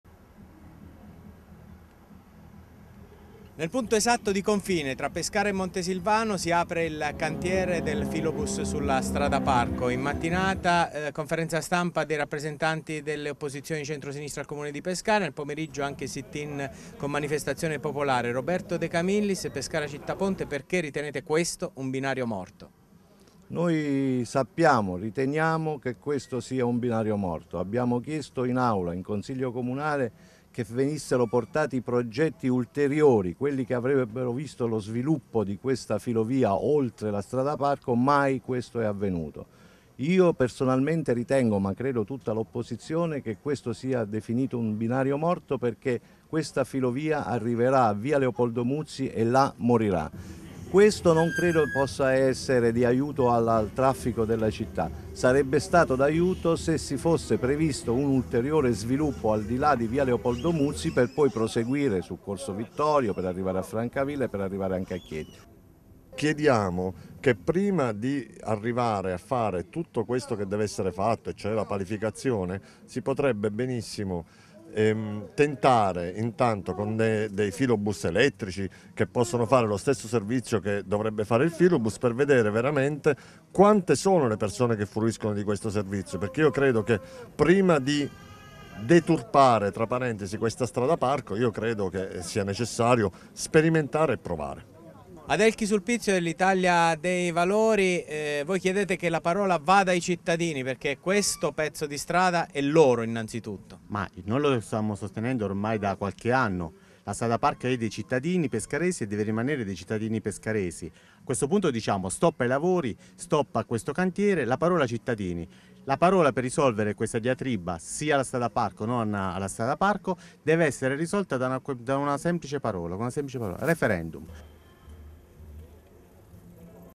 Strada-parco, il "No" non si arrende
La protesta e la proposta delle opposizioni a Pescara sul cantiere del Filobus aperto sulla strada parco. Nel pomeriggio altra manifestazione popolare di protesta